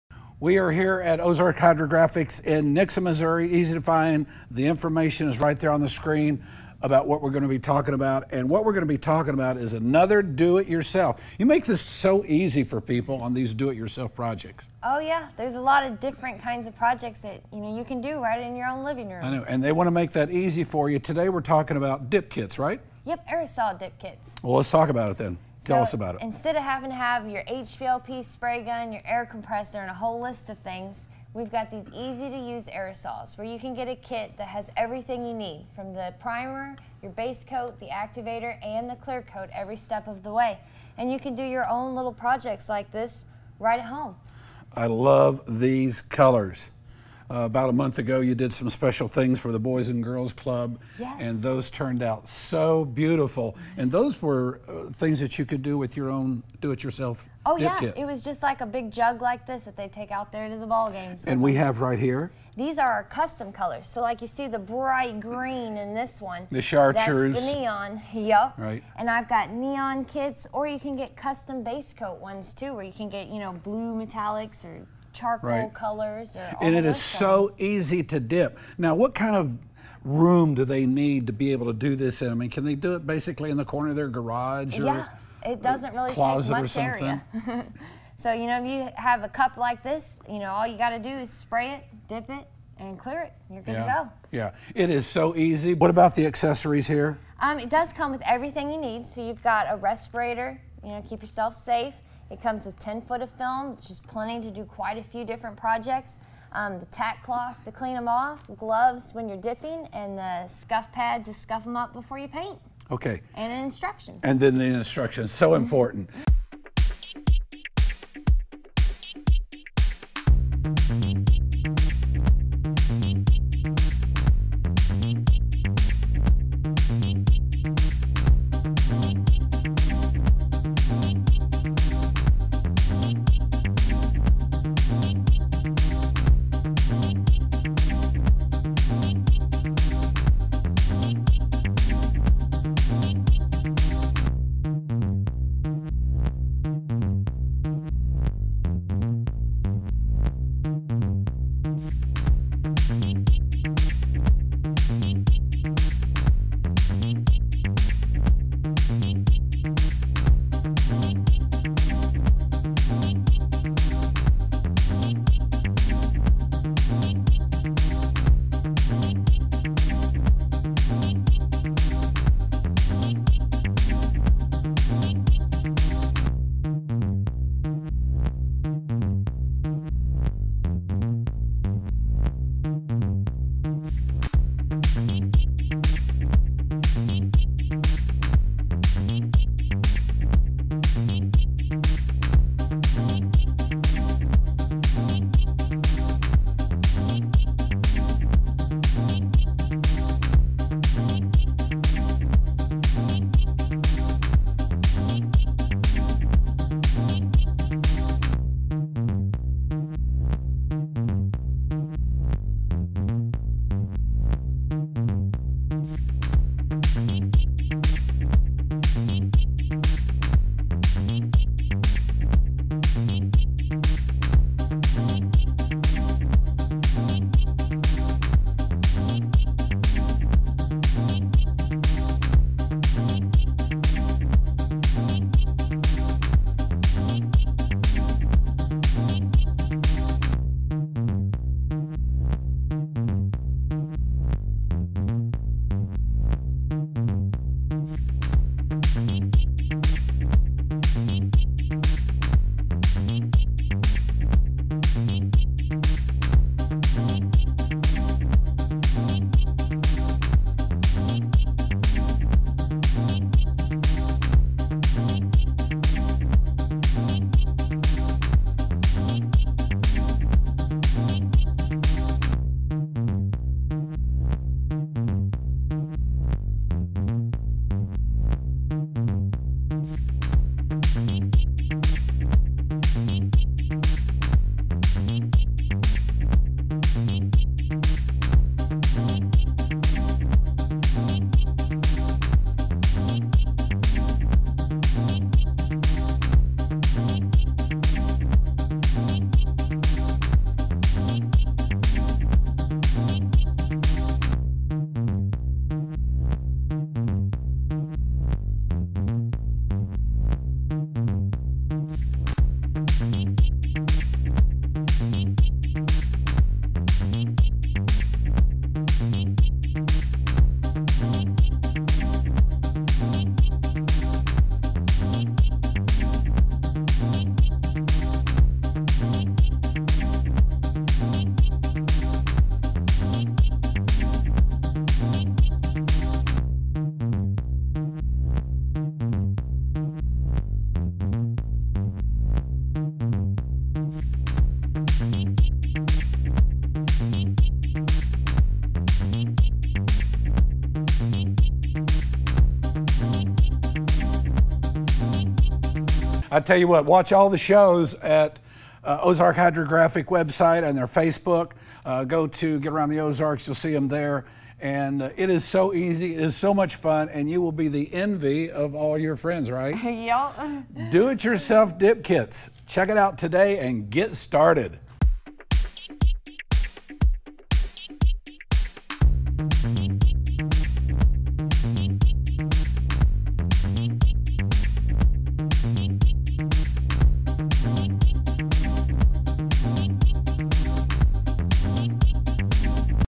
Ozarks Hydrographics Aerosol DIY Dip Kits for Hydrographics Dipping
Check out just how easy it is to do this at home today! Dip your favorite parts to be completely customized! Anything you can paint you can dip which includes guns, motorcycles, skulls, four wheelers, automotive parts, wheels, and much much more!

These DIY Dip Kits include:
Aerosol DIY Dip Kit - Wildfire
Included in this kit is:
1 Primer Gray Aerosol Can
1 White Hydrographics Basecoat Aerosol Can
10' Wildfire Hydrographics Film Pattern
1 Hydrographics Activator Aerosol Can
1 Urethane Clearcoat Gloss Aerosol Can
1 Respirator
5 pack of Gloves
2 Scuff Pads
1 Tack Cloth
DIY Instructions
All Aerosol Cans have a special nozzle that allows the spray pattern to be adjusted, spraying just like a HVLP Spray gun would.

This DIY Dip Kit includes everything you need to get started in Hydrographics. This kit was designed for the DIY community who wants to get into this line of work for a very small investment. You are welcome to call for assistance with dipping anytime during our normal business hours of M-Fri 9am-6pm